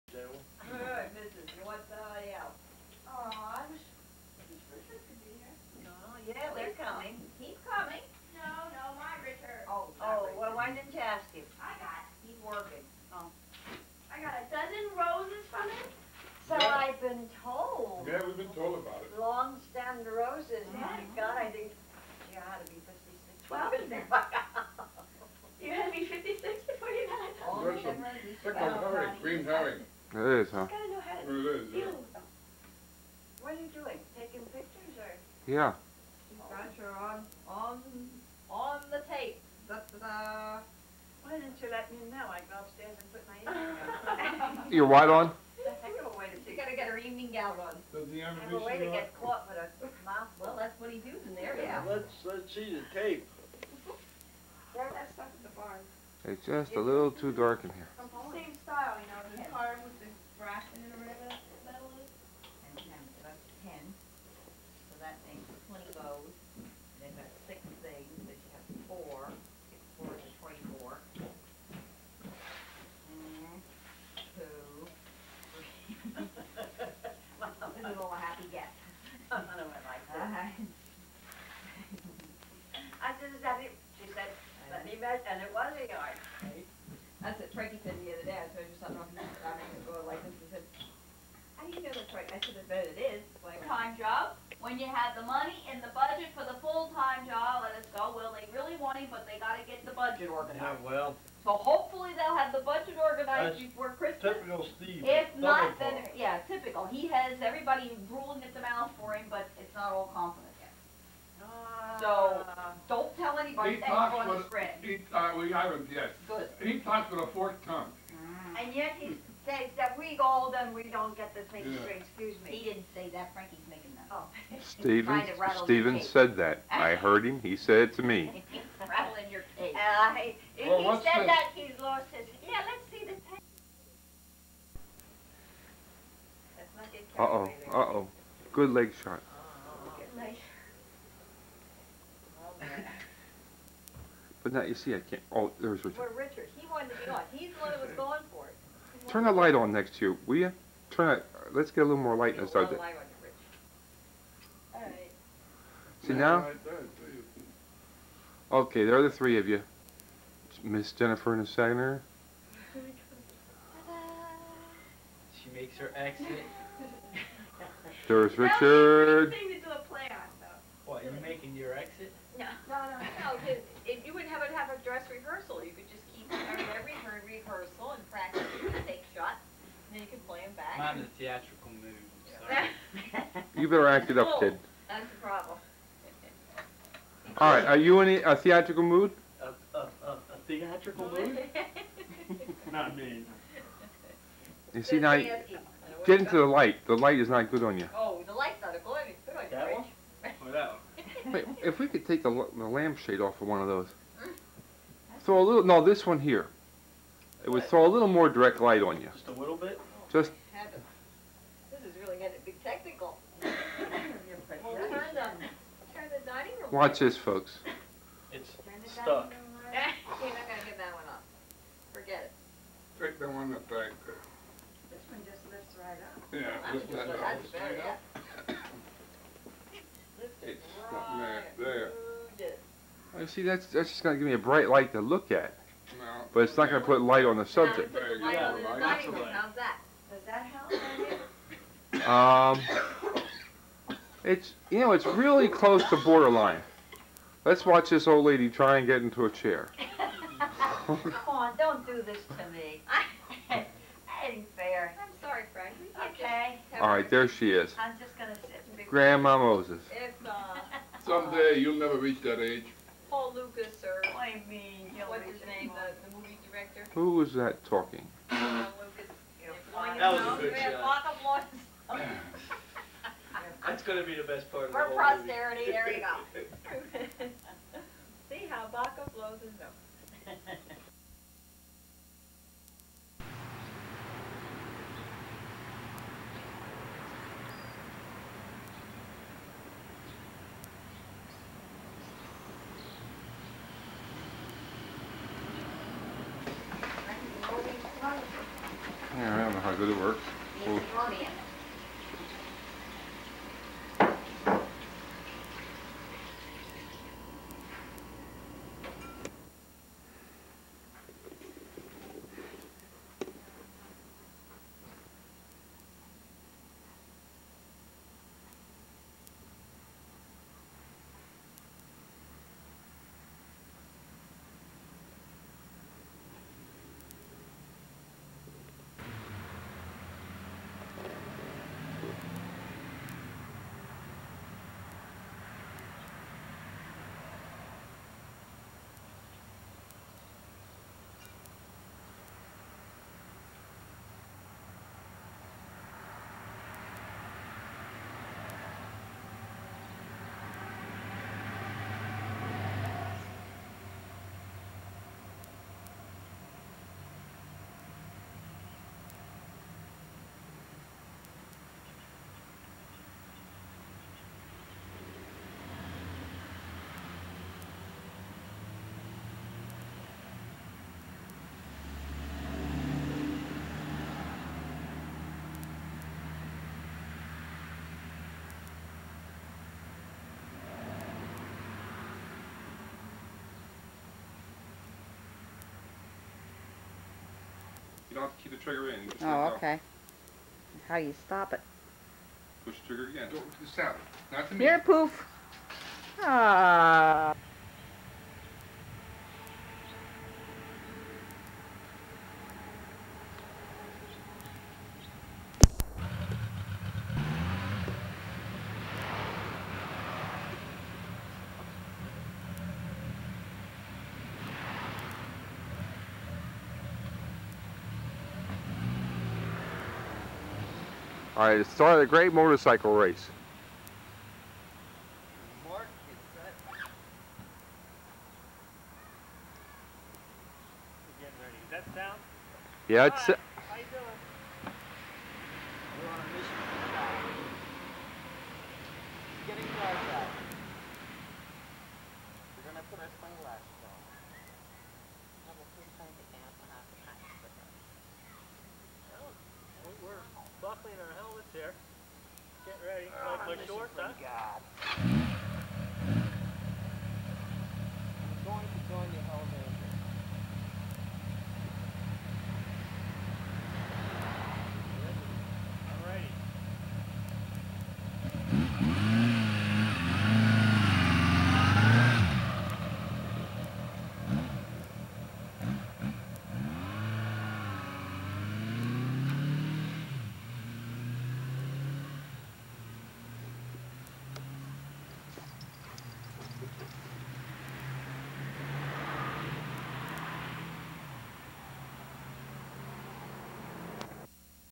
pop vhs 3B
Thanksgiving 1984 at 813 Penndale, Reading, PA

Frank tries out his new video camera

Jennifer Swienckowski @ 00:00
Sally Swienckowski @ 00:00
Frank J. Swienckowski
Eugenia "Jean" Swienckowski
Frank S. Swienckowski
Gerald Bench
David Berman @ 2:17
Richard Berman @ 2:27
Mark Swienckowski @ 7:00

The Christmas Barn